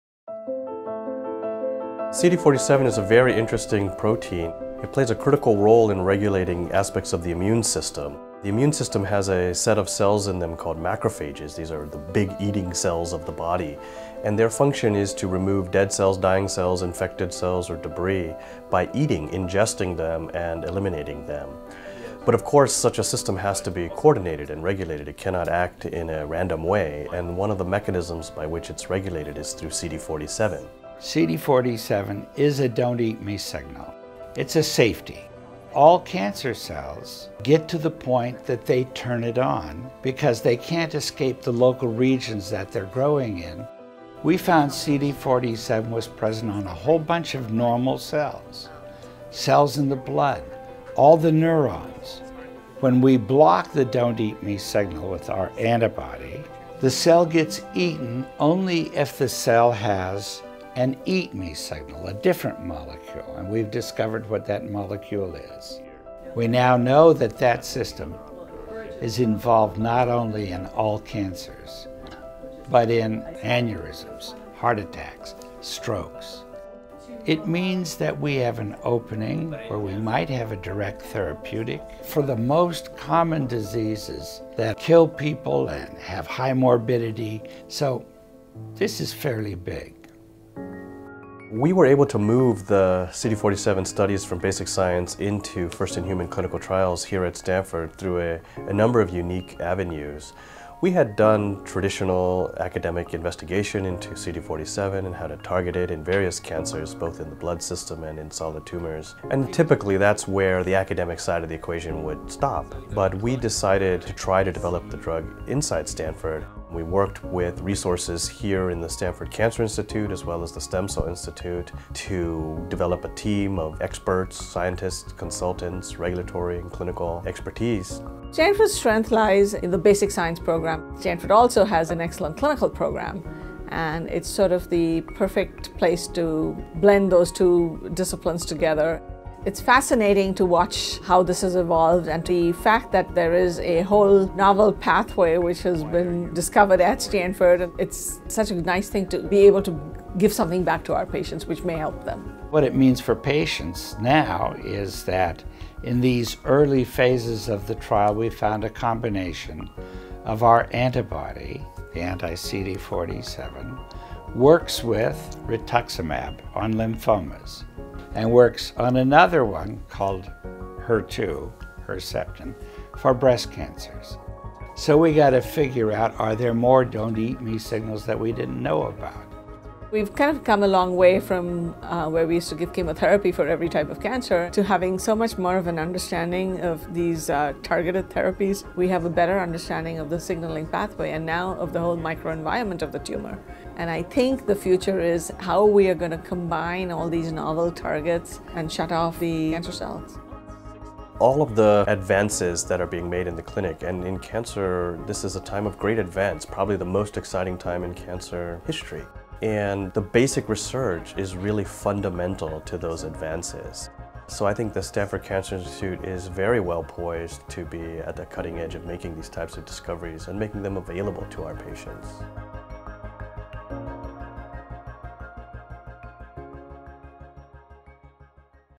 CD47 Discovery: Flipping the Script on Cancer | Stanford Cancer Institute
Cancer cells are covered with a protein called CD47, which acts as a “don’t eat me” signal to the immune system. Stanford researchers developed an anti-CD47 antibody that blocks the CD47 protein, prompting the body's immune system to engulf and devour the cancer cell.